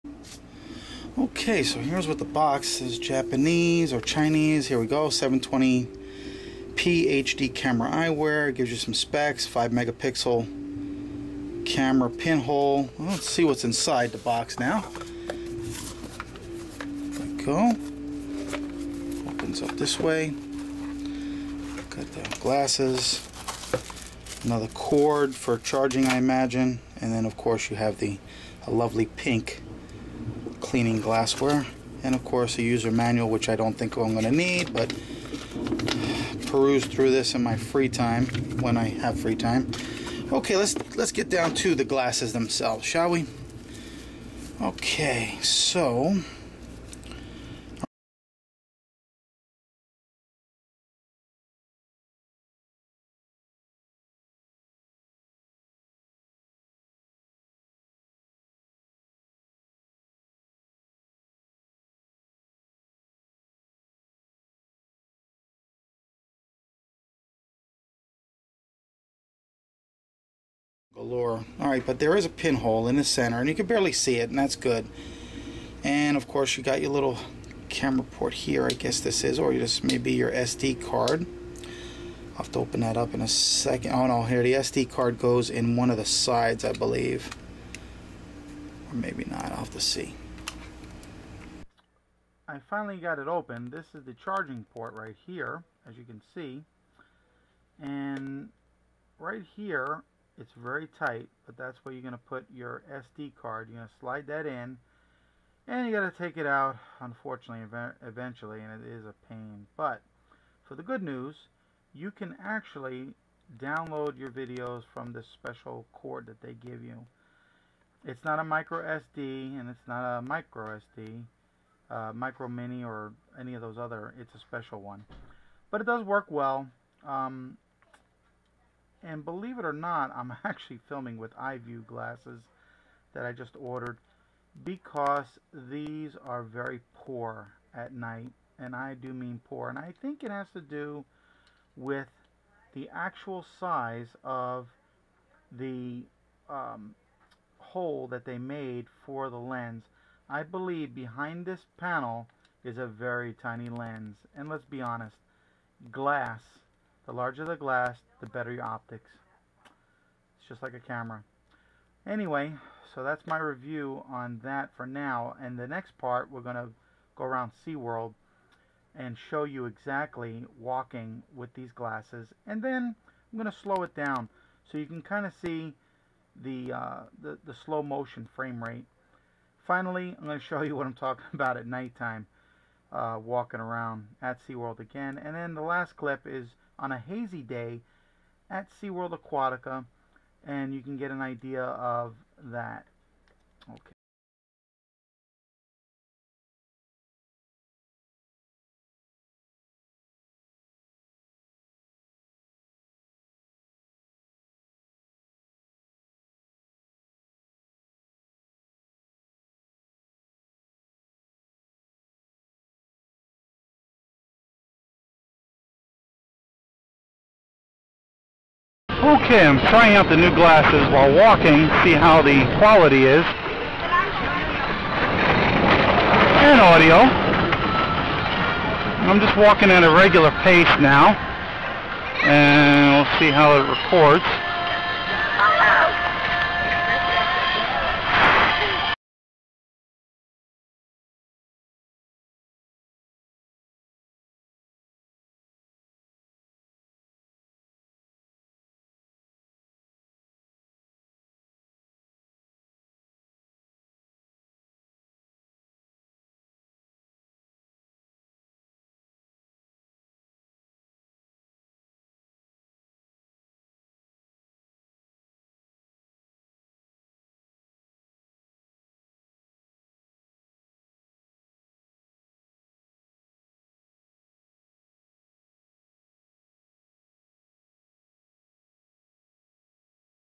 HONEST REVIEW of 720P HD camera eyewear spy camcorder DVR sunglasses video recorder from China
This is my honest review and opinion of these newly designed camera / camcorder spy sunglasses.
     These glasses were the first I had seen on Ebay featuring a yellow tint mirror and an extremely small hole in the middle of the glasses.  I decided to do a testing of these since they only cost me $35.  BTW, you do get what you pay for!
     Cheap? yes   Usable?  yes     Well, these are cheap and they work ok during the day but there are several issues with them.  Watch the video to find out the issues and determine if these would work for you.  For my purpose, I have no choice but to spend $129 minimum to get something that will work for me.  I have seen the reviews for several brands and decided on a much more expansive brand.
     The English manual is a little confusing but they are extremely easy to figure out.  There is only one button and two functions to remember, take a picture or take video.  The key is the power cord that comes with the unit.  Do not lose it or you are doomed.  The audio is not good either but at least there is audio.  Some cheap units do not have audio at all so something is better than nothing.
If you are looking for a pair to try out and have some fun, then these would work for you.